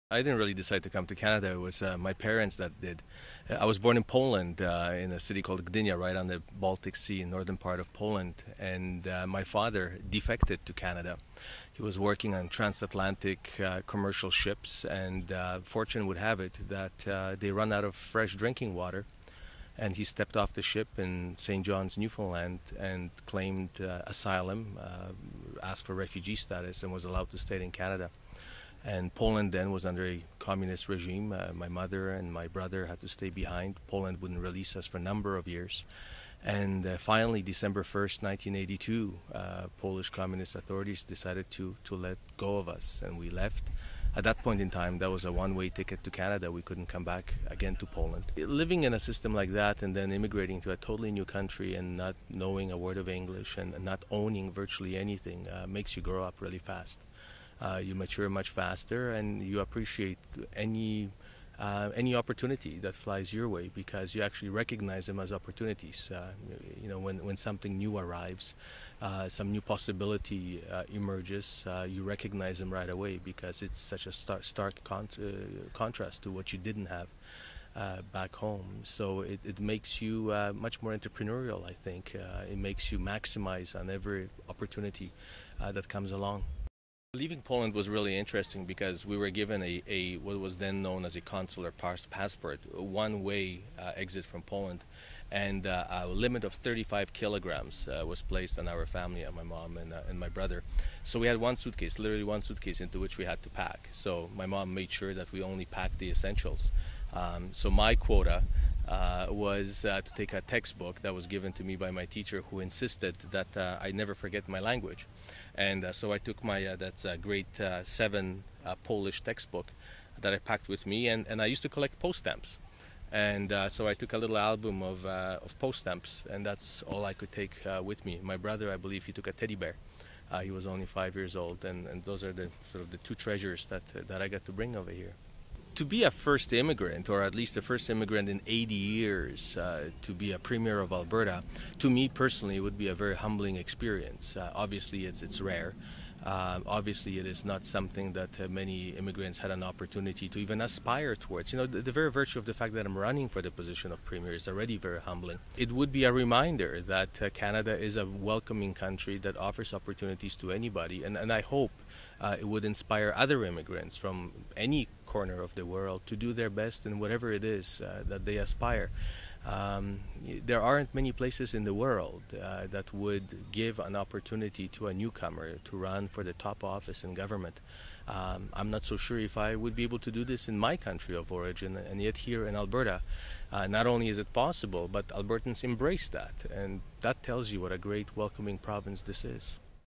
The Life Story of Thomas Lukaszuk - Moving to Canada from Poland
Thomas Lukaszuk talks to New Canadian Media about moving to Canada from Poland.